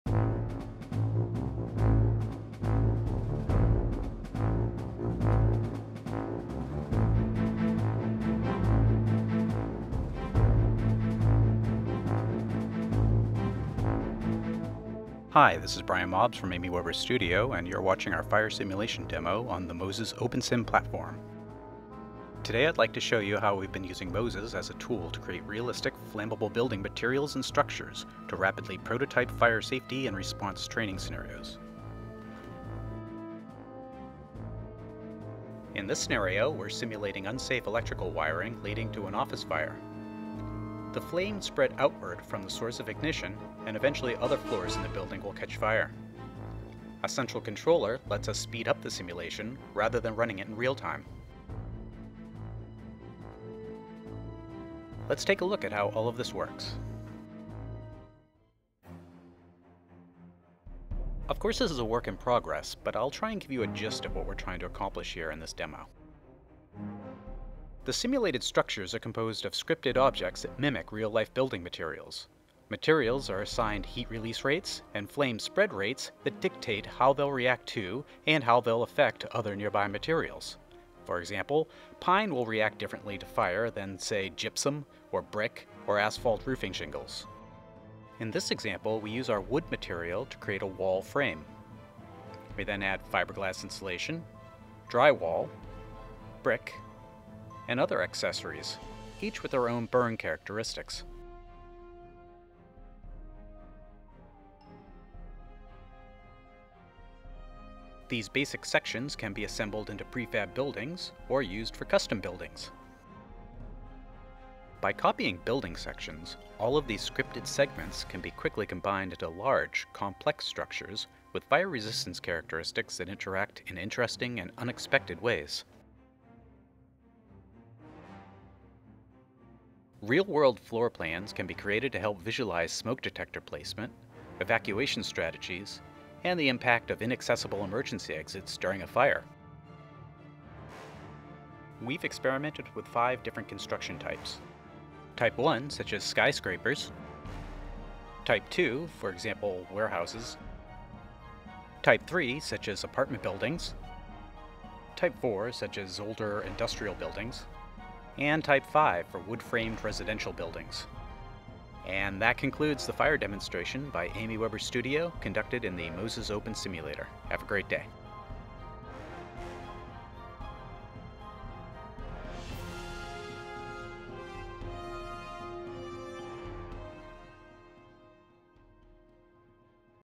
MOSES OpenSimulator Fire Simulation Video
AIMEE WEBER STUDIO for US Army Simulation and Training Technology Center (STTC). This video illustrates scripted fire burning and spreading through materials scripted to mimic real world flammability physical properties.

MOSES (Military Open Simulator Enterprise Strategy) Grid is an OpenSimulator, online virtual environment with security and performance enhancements, open to MOSES Community members, hosted on STTC servers in Orlando, Florida.